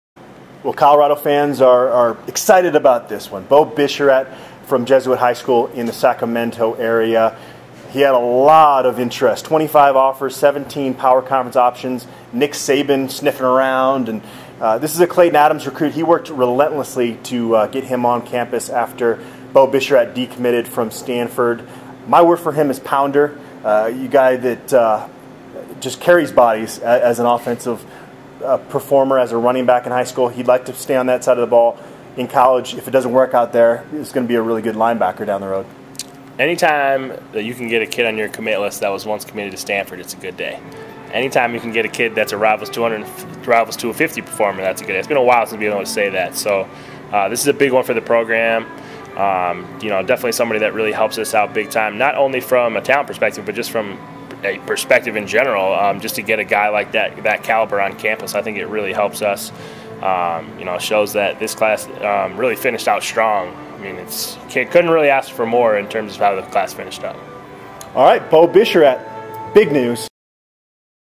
Beau Bisharat commits to Buffs
Carmichael (Calif.) Jesuit athlete Beau Bisharat has signed a letter of intent with Colorado.